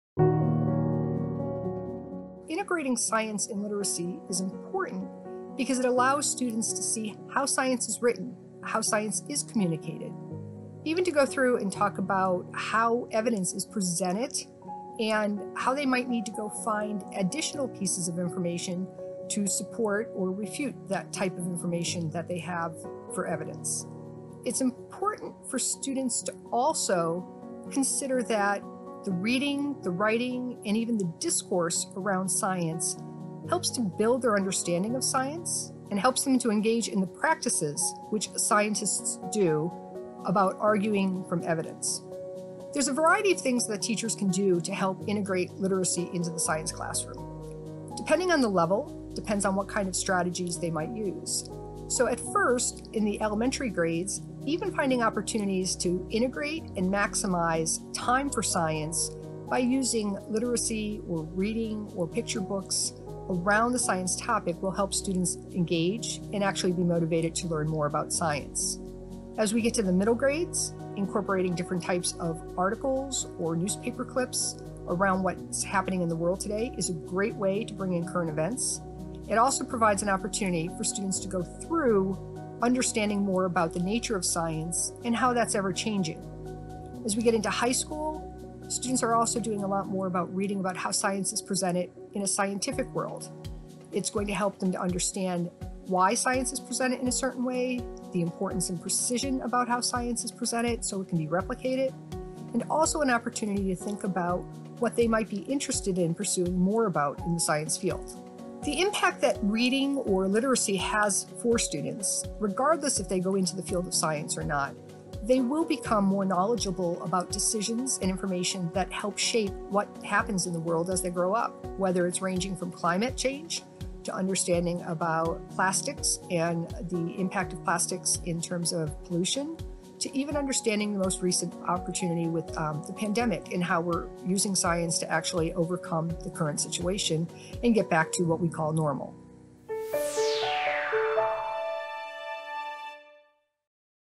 Science and Literacy
Science and literacy go hand in hand. NSTA TV sat down with a former NSTA President about why integrating science and literacy is crucial and how you can implement this into your classroom.